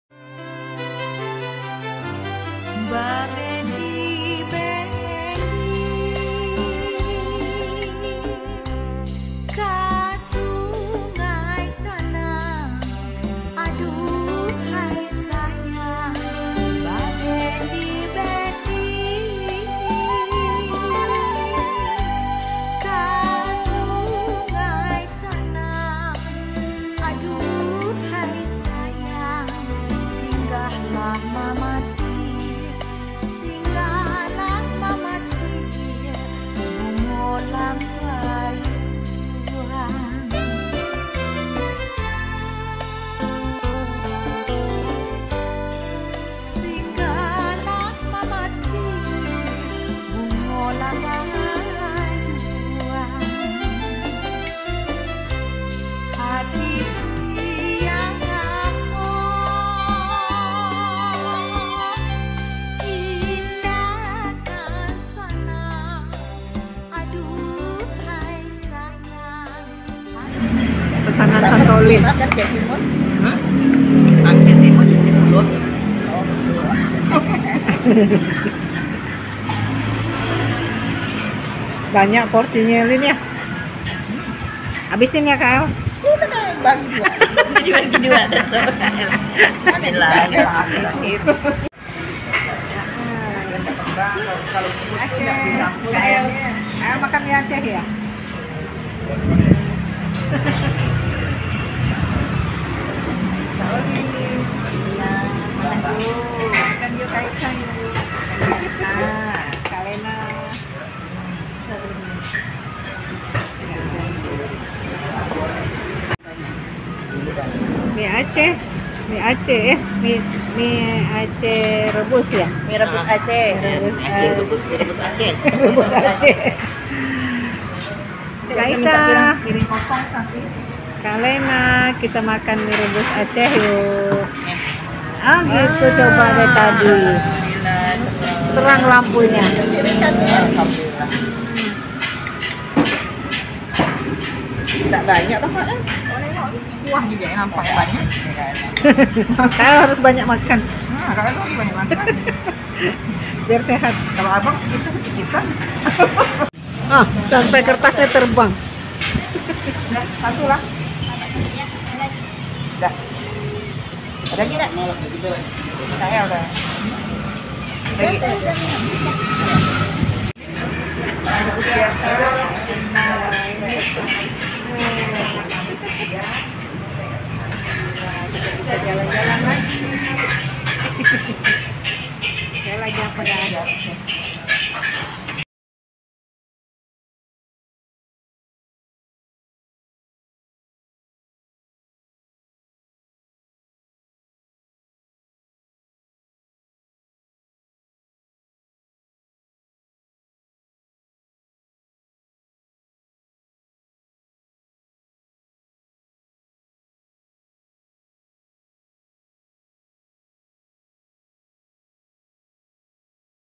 Jalan sore sore menikmati Mie Ace bersama Kak El Olin dan Niniek
9 Desember 2021
Pontianak